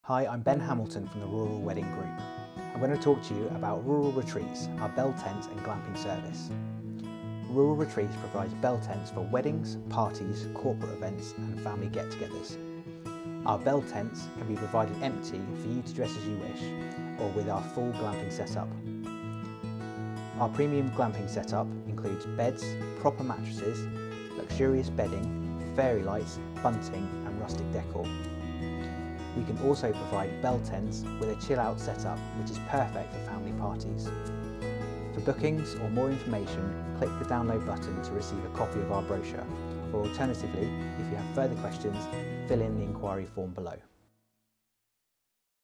Wedding Glamping | Bell Tents | Festival Weddings
A quick look into our wedding glamping accommodation.